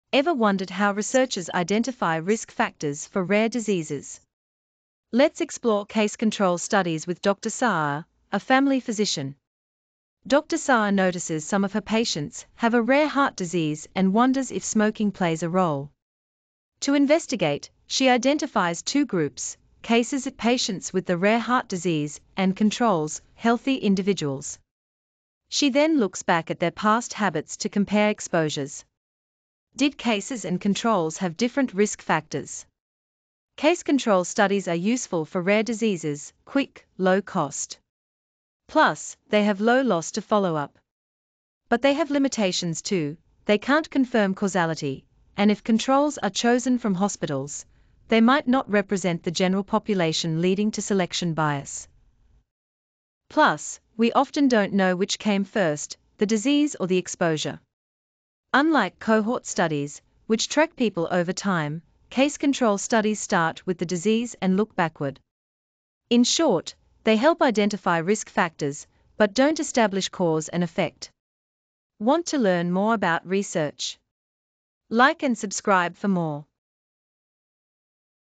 Study design - Case-control studies
Curious about how researchers identify risk factors for rare diseases? This video explains case-control studies using a real-world example from family medicine! Learn how doctors compare patients with and without a disease to uncover potential causes. Watch now to understand the pros, cons, and how this method differs from other study designs!